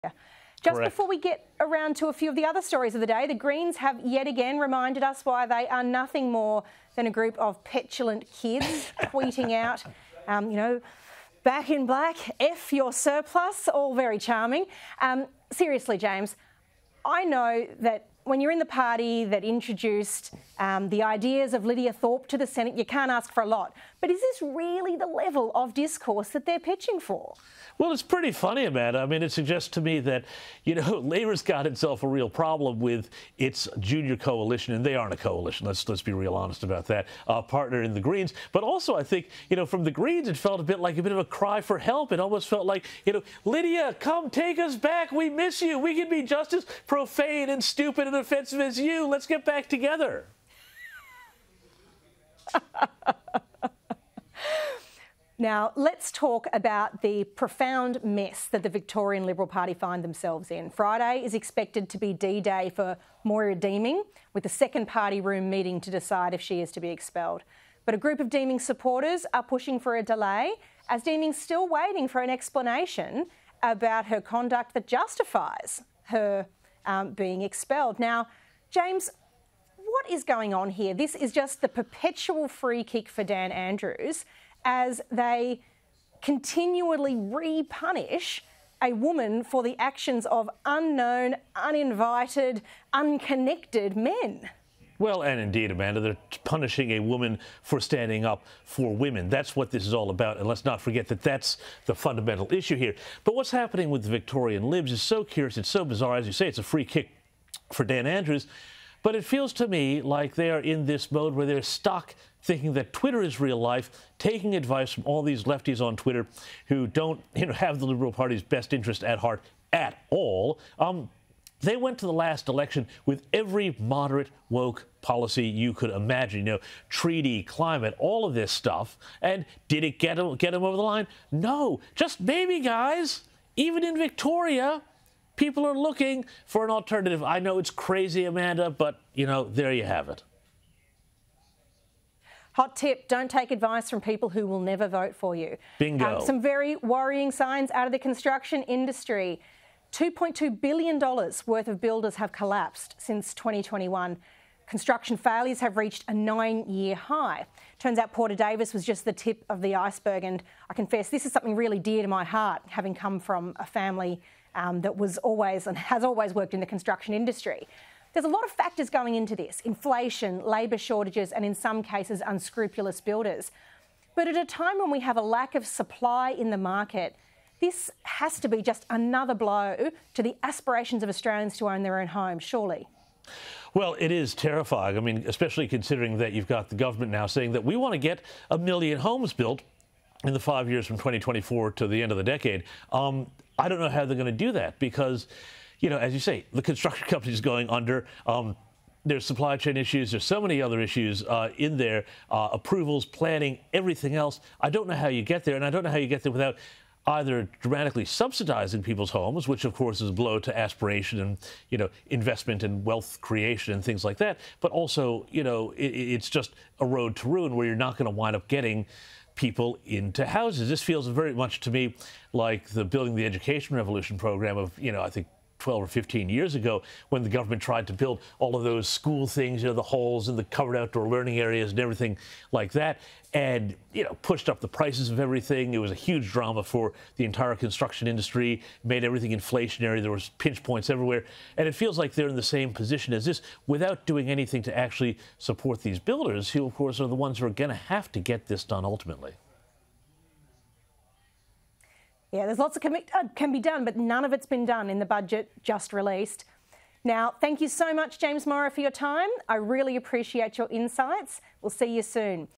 ‘Cry for help’: Greens show they are just as ‘stupid’ as Lidia Thorpe
Sky News host James Morrow says the Greens’ reaction to the budget shows they are just as “profane” and “stupid” as Lidia Thorpe.

In response to the Labor government’s new budget, the official Greens Twitter account posted a photo reading “f*** your surplus”.

“It felt a bit like a cry for help,” Mr Morrow told Sky News host Amanda Stoker.

“Labor has got itself a real problem with its junior coalition and they aren’t a coalition.”